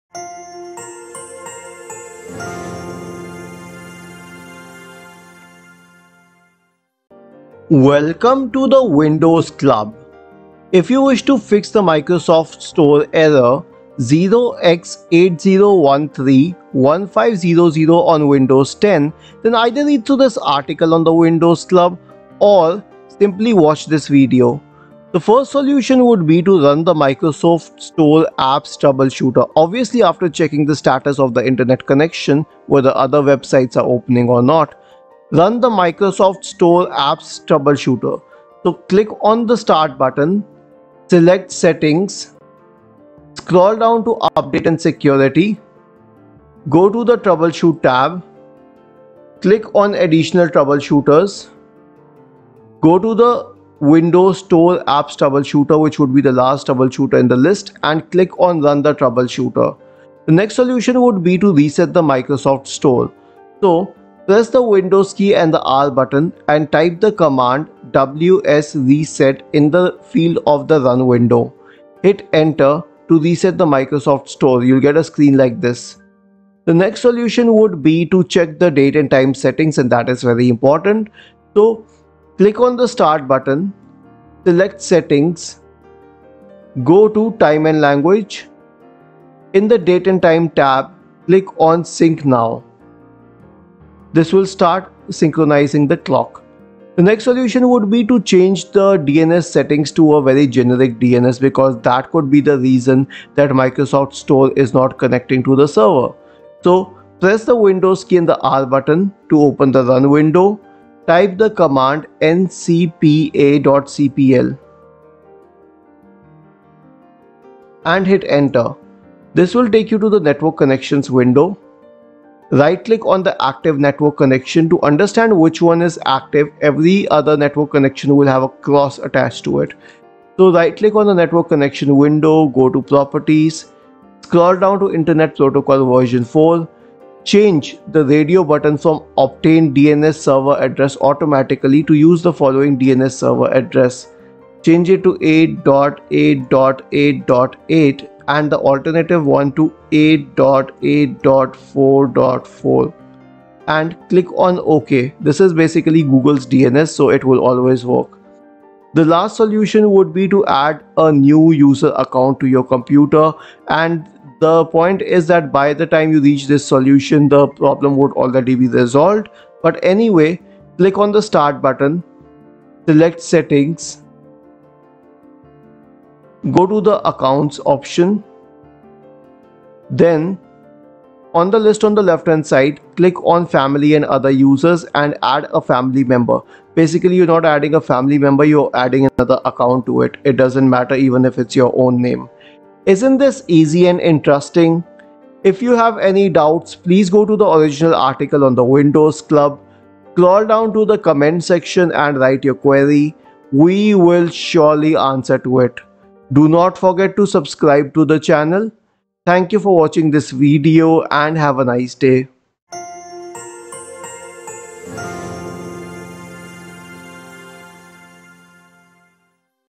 Fix Microsoft Store error 0x80131500 on Windows PCC
The Microsoft Store is an integral part of the Windows operating system. It is the distribution hub of various applications like Facebook, Netflix, and more. Microsoft is looking forward to making it a hub for UWP applications and Windows Classic Applications. With the power of Project Centennial, Microsoft has built a bridge that will let developers migrate and upload their Windows Classic applications to the Microsoft Store. Spotify is one example of this. However, users often report facing error 0x80131500 while opening the Microsoft Store.

Try that again, Something happened at our end, Waiting a bit might help, The error code is 0x80131500 in case you need it.

We will be taking a look at the different ways you can follow to fix the error 0x80131500 for Microsoft Store on Windows 11/10:

1] Change the internet connection and see

2] Run Microsoft Store Apps Troubleshooter

3] Reset the Microsoft Store

4] Toggle the Date and Time Settings

5] Change DNS to another like OpenDNS

6] Create a new User Account on your computer

7] Use Windows PowerShell